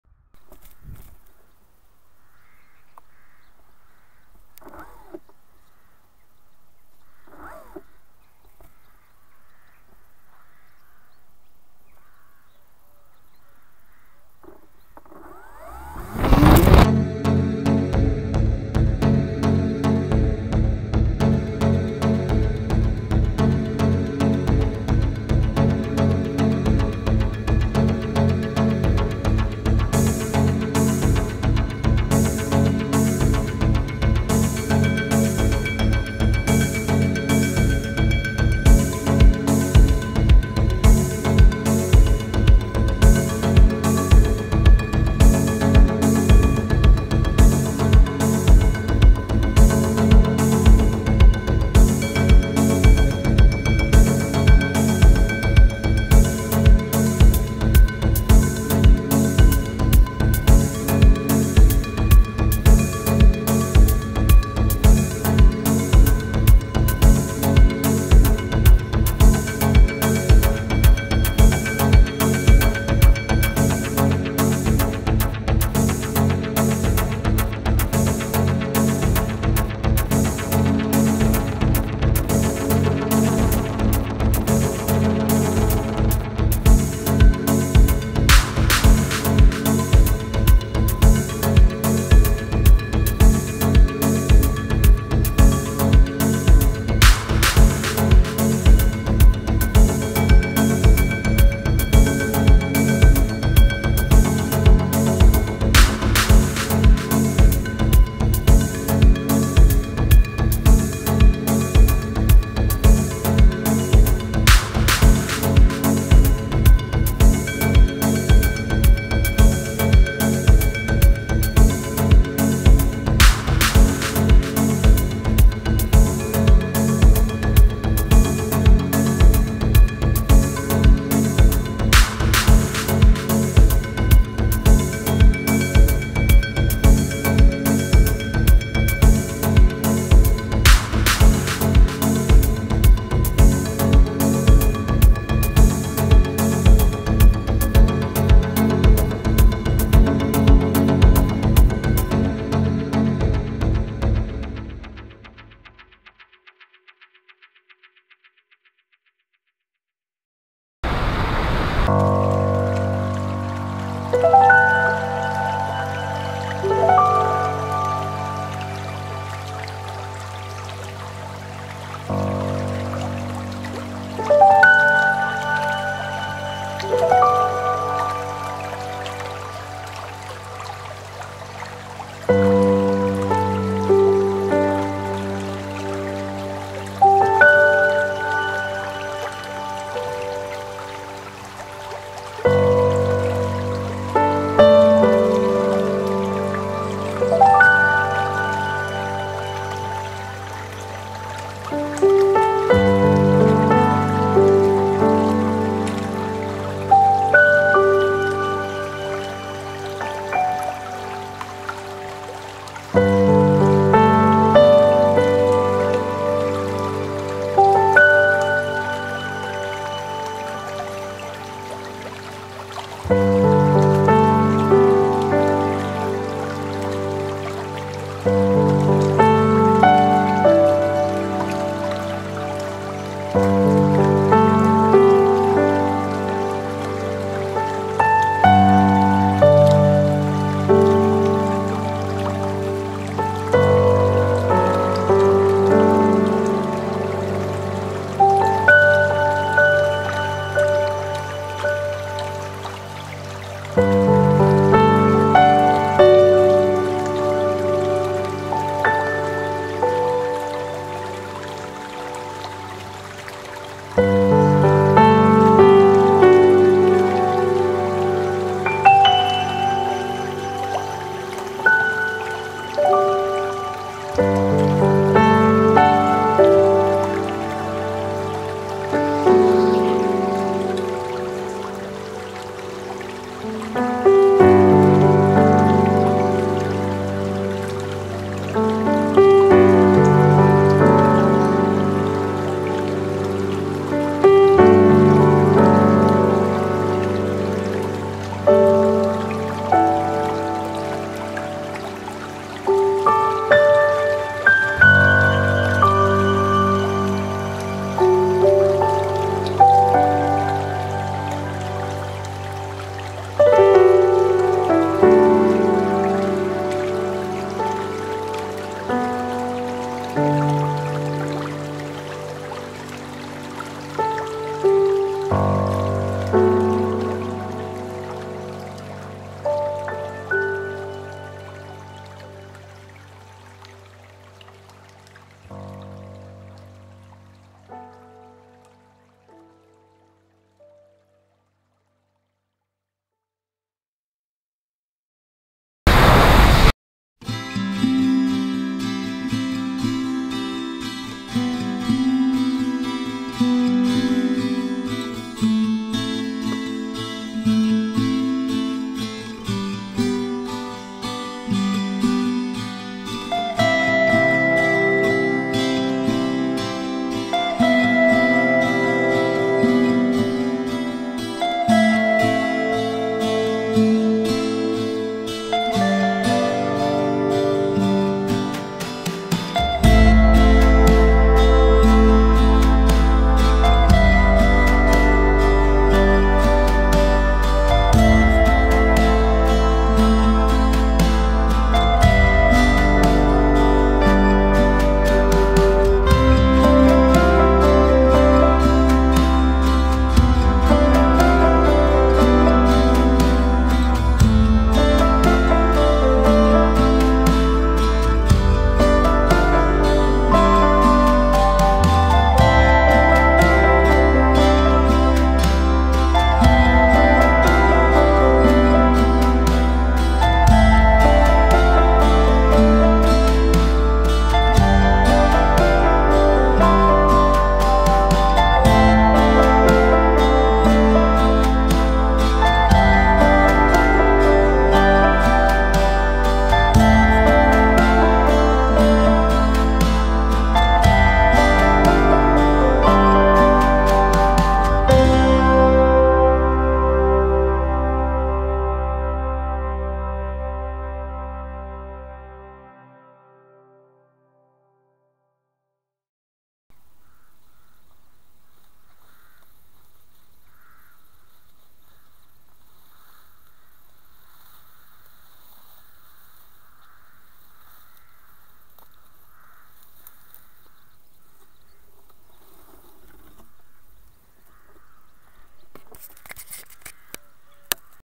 Short flight of the ASW 28, landing in the barley.
Short evening flight. The swallows and crows disapproved of it though...